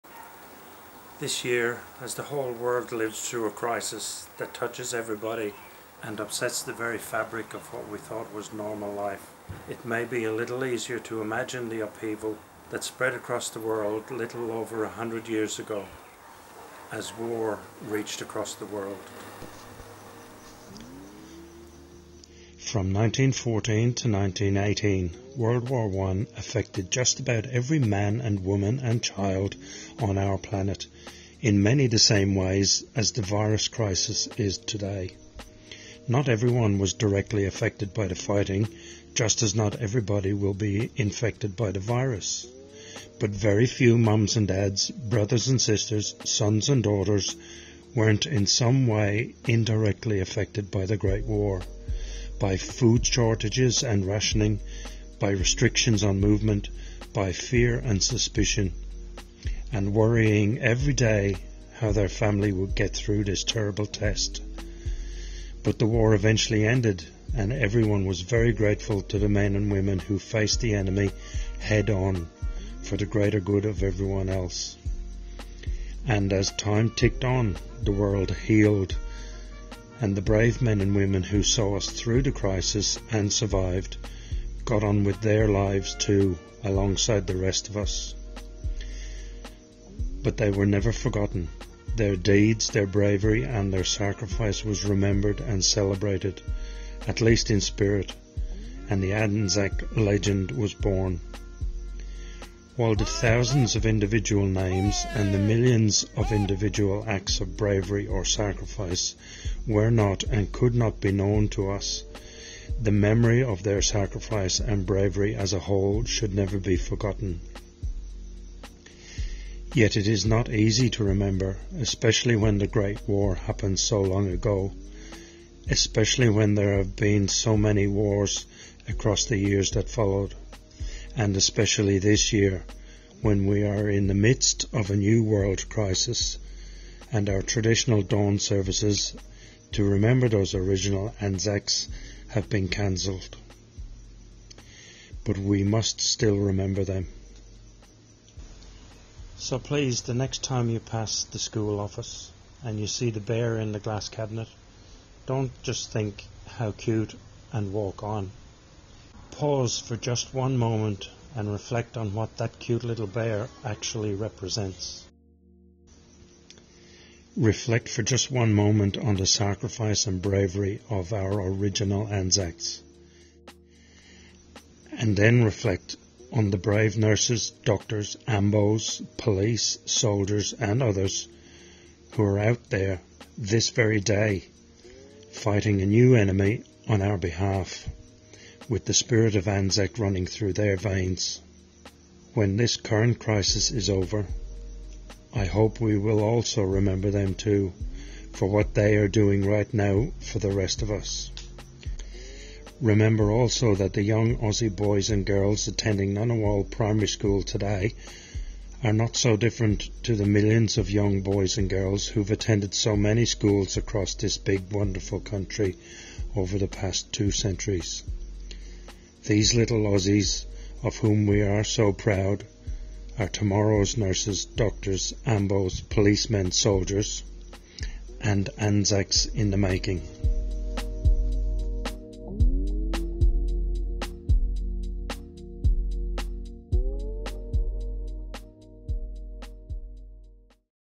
ANZAC's in the making
I made an ANZAC Day video for my granddaughter Hayley's Ngunnawal Primary School – and I might as well share it here too.

Ngunnawal Primary School said...
We are fortunate to have a message from Brian Hartigan, an ex Army Sergeant and an NPS grandad to share with our community. Be sure to spend a few minutes reflecting on Brian’s message to our young ANZACs in the making, our students. A big thank you to Brian for taking the time to create an opportunity for all Australians to commemorate ANZAC Day 2020.